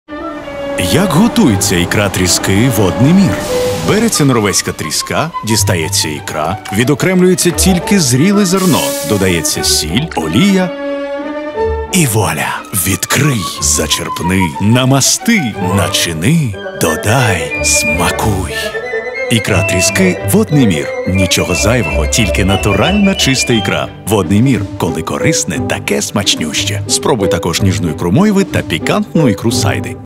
Ікра тріски "Водный Мир" - Коли корисне таке смачнюще!, 30 сек
Ікра тріски «Водный Мир» - нічого зайвого, тільки натуральні чиста ікра.